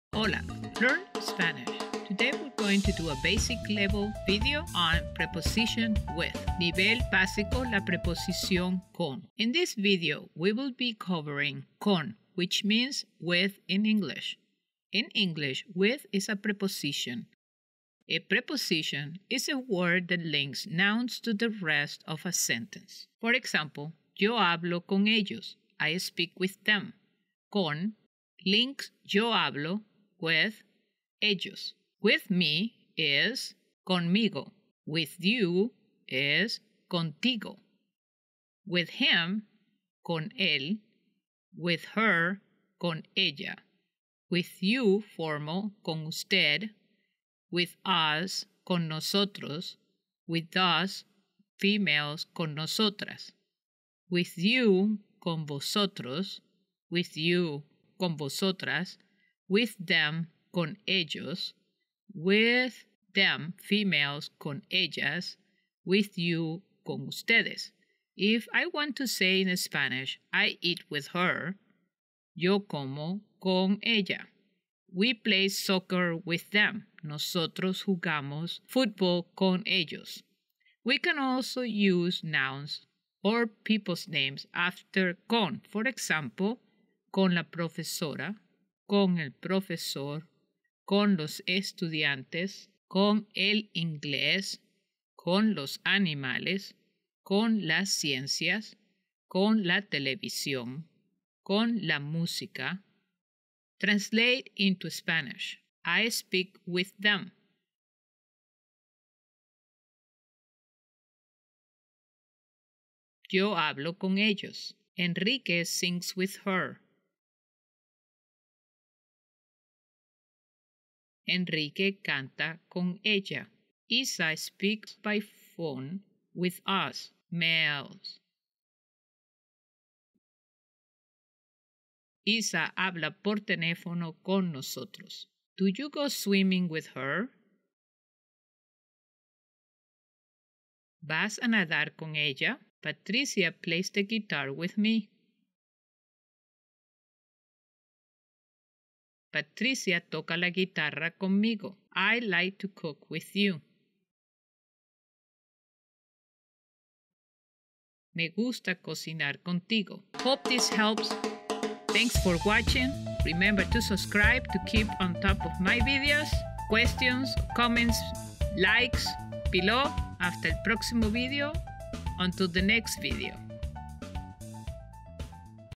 Learn Spanish- How to Use the Preposition «With» in Spanish
Aprendiendo Español- Cómo Usar La Preposición «Con»
In this video, we are going to do a Basic Level with preposition «With». A preposition is a word that links nouns to the rest of a sentence. For example:
Yo hablo con ellos. (I speak with them.)
con (with) links yo hablo with ellos
(with me)                conmigo
(with you)               contigo
Notice the spelling on these two: conmigo and contigo
(with him)                               con él
(with her)                               con ella
(with us- males)                      con nosotros
(with us- females)                   con nosotras
(with you-males)                     con vosotros
(with you- females)                 con vosotras
(with them-)                            con ellos
(with them-females)                con ellas
(with them-formal)                  con ustedes

If I want to say in Spanish:
Yo como con ella
(I eat with her.)
Nosotros jugamos fútbol con ellos.
(We play soccer with them.)

We can also use nouns or people's names after «con». For example:
con la profesora
con el profesor
con los estudiantes
con el inglés
con los animales
con las ciencias
con la televisión
con la música
Translate into Spanish:
1. I speak with them.
2. Enrique sings with her.
3. Isa speaks with us by phone.
4. Do you go swimimg with her?
5. Patricia plays the guitar with me.
6. I like to cook with you.